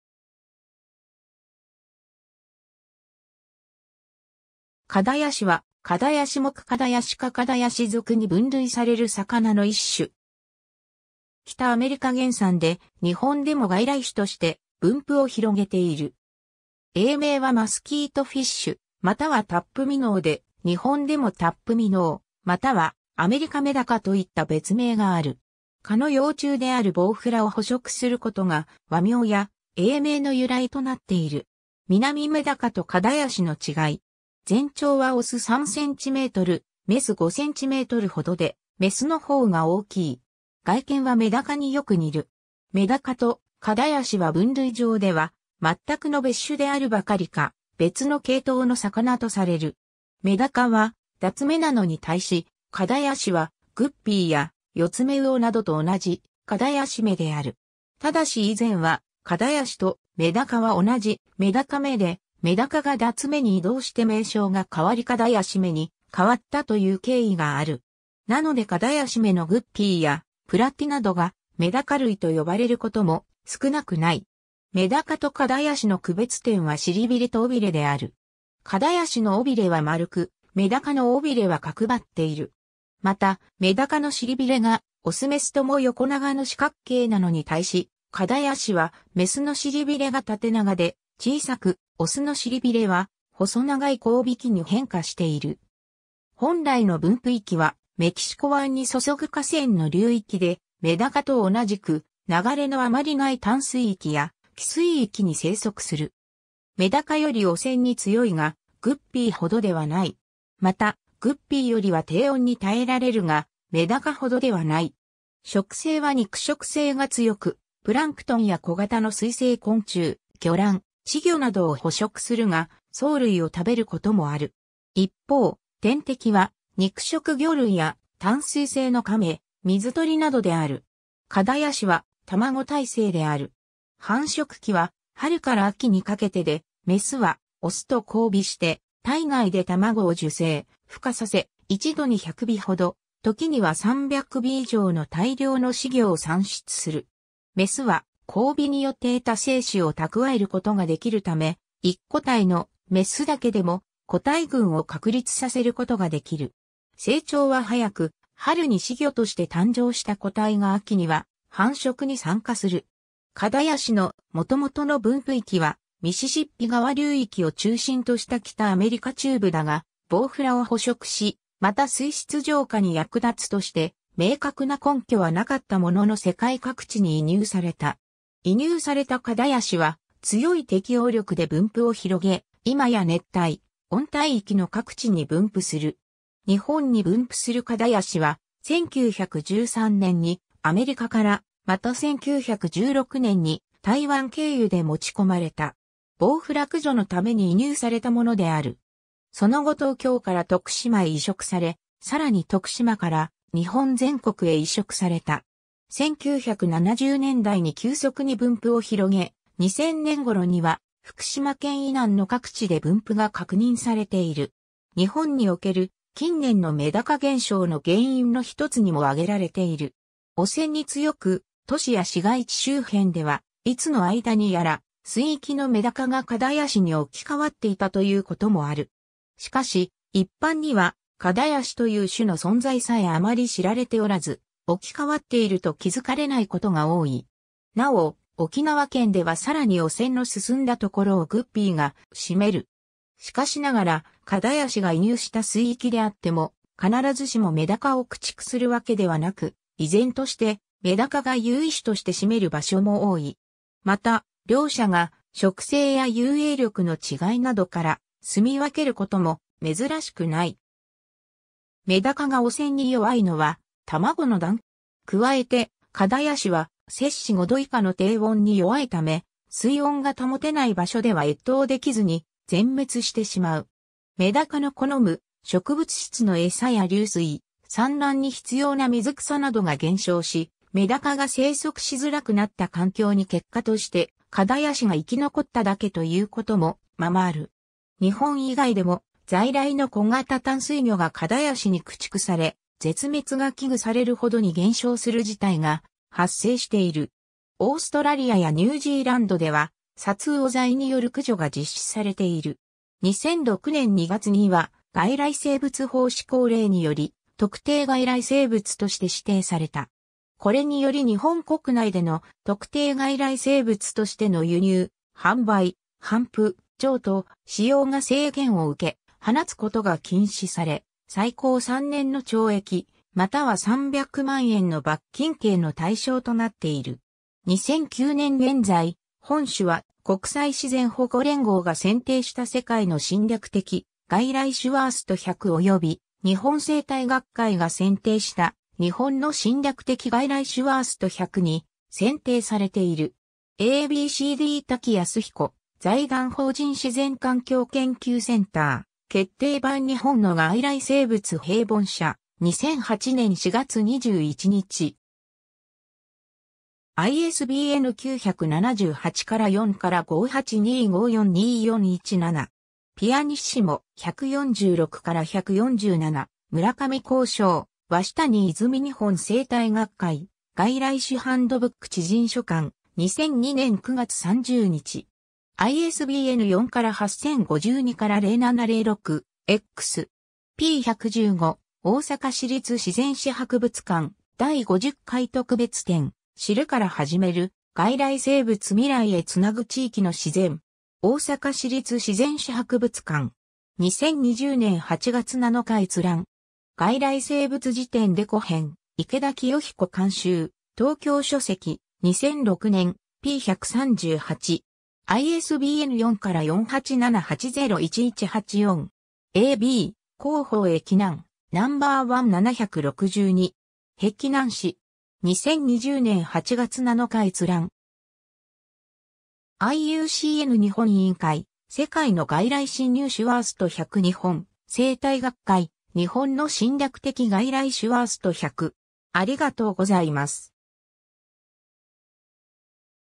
カダヤシ
カダヤシ（蚊絶やし、学名：Gambusia affinis）は、カダヤシ目カダヤシ科カダヤシ属に分類される魚の一種。
北アメリカ原産で、日本でも外来種として分布を広げている。
 英名は Mosquitofish または Topminnow で、日本でもタップミノー、またはアメリカメダカといった別名がある。
蚊の幼虫であるボウフラを捕食する（蚊絶やしする）ことが、和名や英名の由来となっている。
 ミナミメダカとカダヤシの違い。
 全長はオス3cm、メス5cmほどで、メスのほうが大きい。
外見はメダカによく似る。
メダカとカダヤシは分類上では全くの別種であるばかりか、別の系統の魚とされる。
メダカはダツ目なのに対し、カダヤシはグッピーやヨツメウオなどと同じカダヤシ目である。
 ただし以前はカダヤシとメダカは同じメダカ目でメダカがダツ目に移動して名称が変わりカダヤシ目に変わったという経緯がある。
なのでカダヤシ目のグッピーやプラティなどがメダカ類と呼ばれることも少なくない。
 メダカとカダヤシの区別点は尻びれと尾びれである。
カダヤシの尾びれは丸く、メダカの尾びれは角ばっている。
また、メダカの尻びれがオスメスとも横長の四角形なのに対し、カダヤシはメスの尻びれが縦長で小さく、オスの尻びれは細長い交尾器に変化している。
 本来の分布域はメキシコ湾に注ぐ河川の流域で、メダカと同じく、流れのあまりない淡水域や汽水域に生息する。
メダカより汚染に強いが、グッピーほどではない。
また、グッピーよりは低温に耐えられるが、メダカほどではない。
 食性は肉食性が強く、プランクトンや小型の水生昆虫、魚卵、稚魚などを捕食するが、藻類を食べることもある。
一方、天敵は肉食魚類や淡水性のカメ、水鳥などである。
 カダヤシは卵胎生である。
繁殖期は春から秋にかけてで、メスはオスと交尾して体内で卵を受精・孵化させ、一度に100尾ほど、時には300尾以上の大量の仔魚を産出する。
雌は交尾によって得た精子を蓄えることができるため、1個体の雌だけでも個体群を確立させることができる。
成長は早く、春に仔魚として誕生した個体が秋には繁殖に参加する。
 カダヤシのもともとの分布域はミシシッピ川流域を中心とした北アメリカ中部だが、ボウフラ（カの幼虫）を捕食し、また水質浄化に役立つとして、明確な根拠はなかったものの世界各地に移入された。
移入されたカダヤシは強い適応力で分布を広げ、今や熱帯・温帯域の各地に分布する。
 日本に分布するカダヤシは、1913年にアメリカから、また1916年に台湾経由で持ちこまれた。
ボウフラ駆除のために移入されたものである。
その後東京から徳島へ移植され、さらに徳島から日本全国へ移植された。
1970年代に急速に分布を広げ、2000年頃には福島県以南の各地で分布が確認されている。
 日本における近年のメダカ減少の原因の一つにも挙げられている。
汚染に強く、都市や市街地周辺ではいつの間にやら水域のメダカがカダヤシに置き換わっていたということもある。
しかし、一般にはカダヤシという種の存在さえあまり知られておらず、置き換わっていると気づかれないことが多い。
なお、沖縄県ではさらに汚染の進んだところをグッピーが占める。
 しかしながらカダヤシが移入した水域であっても、必ずしもメダカを駆逐するわけではなく、依然としてメダカが優位種として占める場所も多い。
また、両者が食性や遊泳力の違いなどから、棲み分けることも珍しくない。
メダカが汚染に弱いのは卵の段階であり、成魚においては両者とも汚染への耐性は大差がない。
加えてカダヤシは5℃以下の低温に弱いため、水温が保てない場所では越冬できずに全滅してしまう。
メダカの好む植物質の餌や流水、産卵に必要な水草などが減少し、メダカが生息しづらくなった環境に結果としてカダヤシが生き残っただけと言うこともままある。
 日本以外でも在来の小型淡水魚がカダヤシに駆逐され、絶滅が危惧されるほどに減少する事態が発生している。
オーストラリアやニュージーランドでは、殺魚剤による駆除が実施されている。
 2006年2月には外来生物法施行令により、特定外来生物として指定された。
これにより日本国内での、特定外来生物としての輸入、販売、頒布、譲渡、飼養が制限を受け、放つことが禁止され、最高三年の懲役、または三百万円の罰金刑の対象となっている。
 2009年現在、本種は国際自然保護連合(IUCN)が選定した世界の侵略的外来種ワースト10...